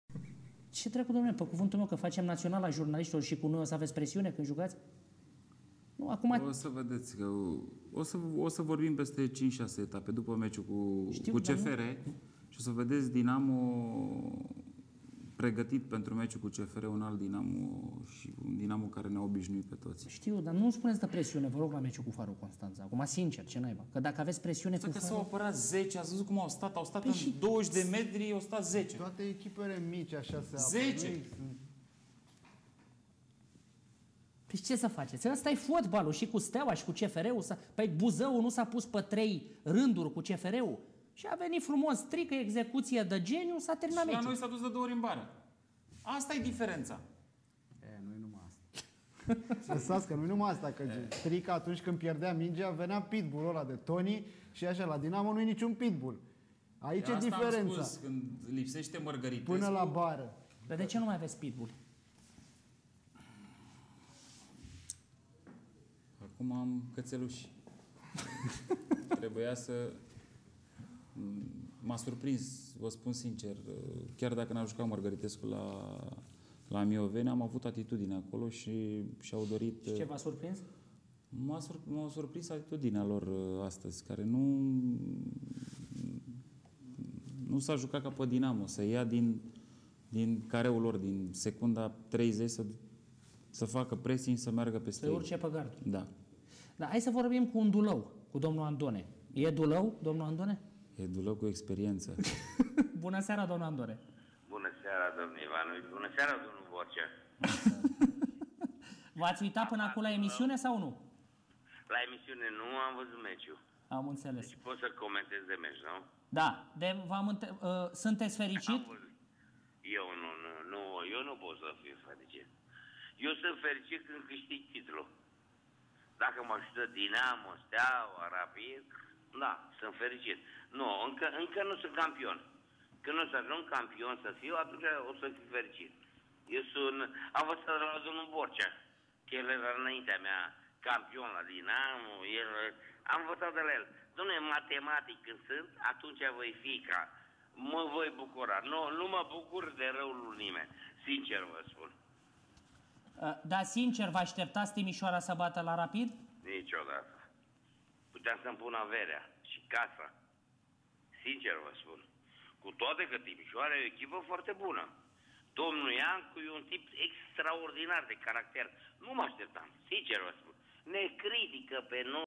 Cristi Borcea dupa meciul cu Farul Constanta
Suparat foc, Cristi Borcea da vina pe apararea Farului ca a fost prea exagerata, gen "10 jucatori pe 20 de m". :)